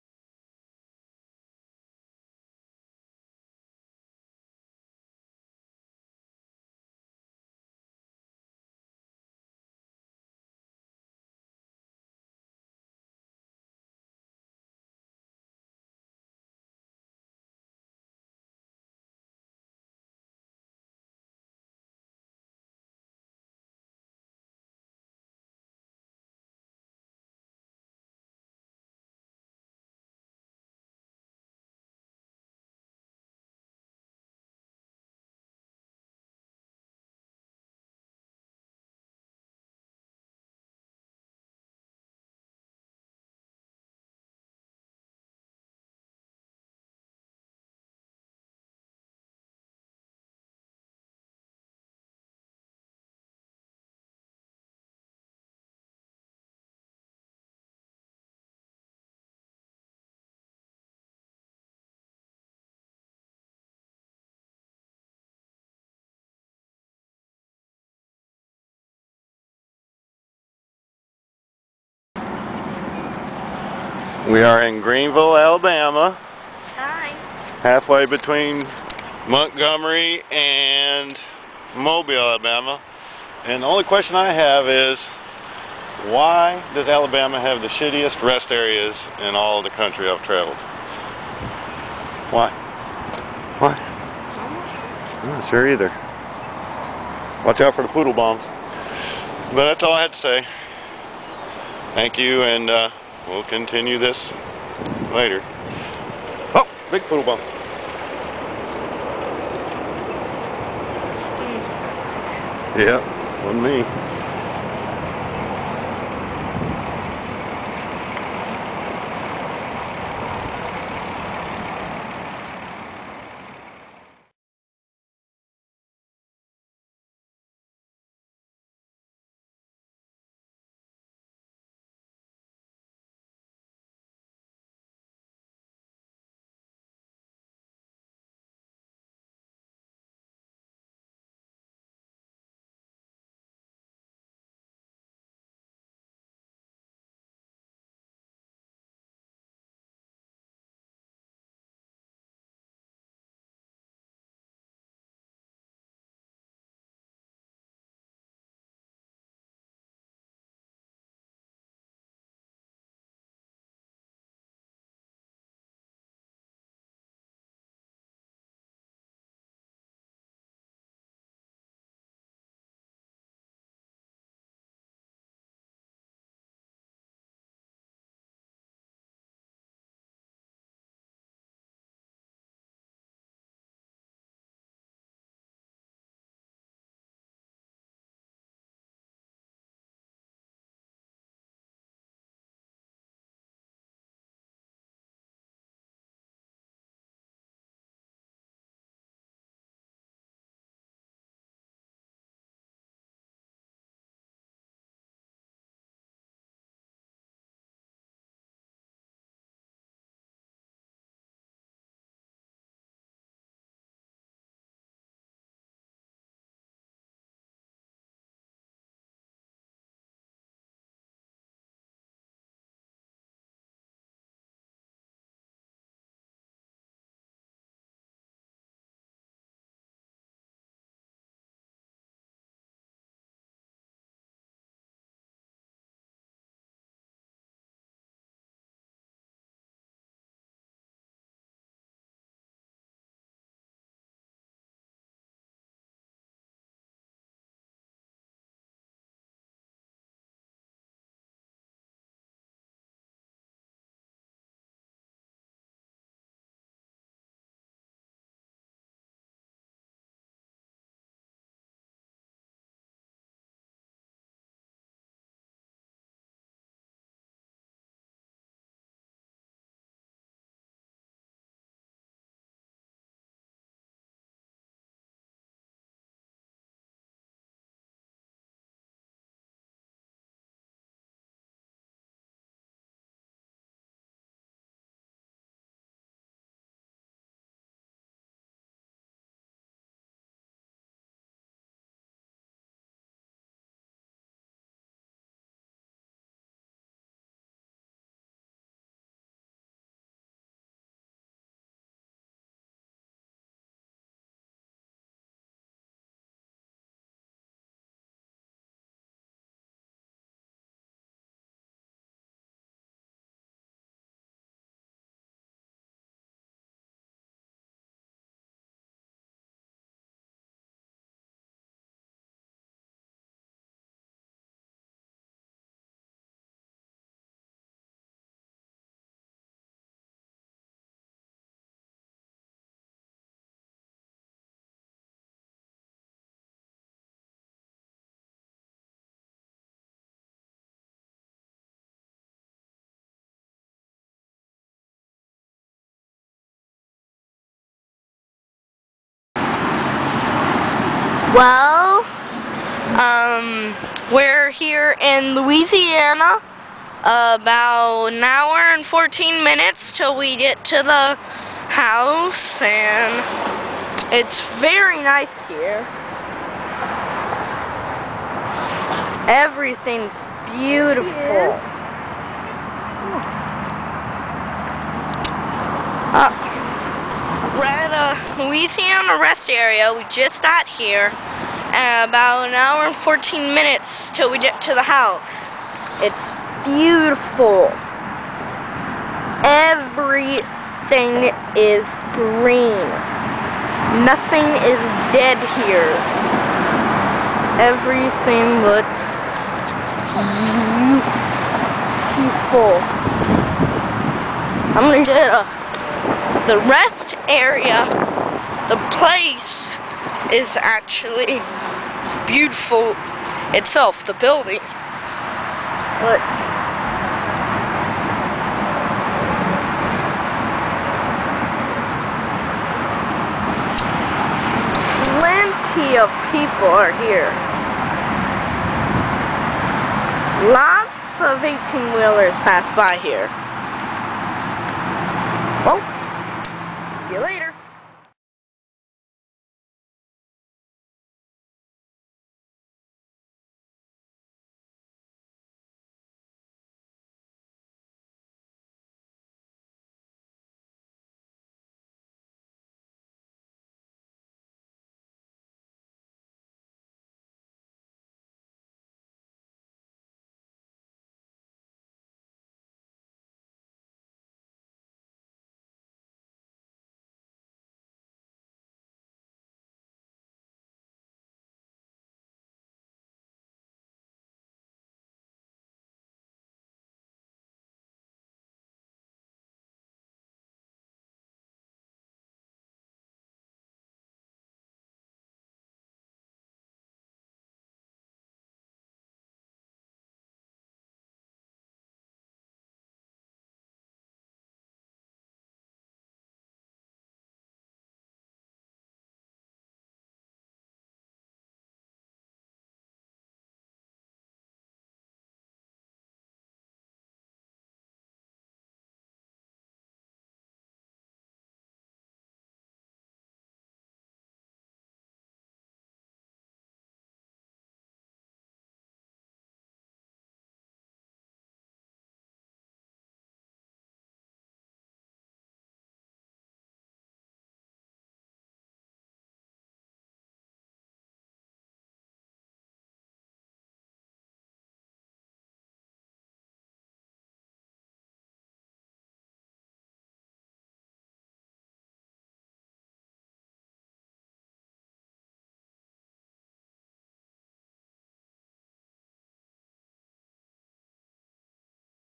January 05 2012 New Year Road Tip II - LaGrange, Georgia to New Orleans, Louisiana
Traveling with my Nephew, Jaq Excho - Son of the Artist Peter Excho - from Fort Bragg, North Carolina to Baton Rouge Louisiana.  This video covers the 2nd day of this road trip, from LaGrange, Georgia to New Orleans, Louisiana.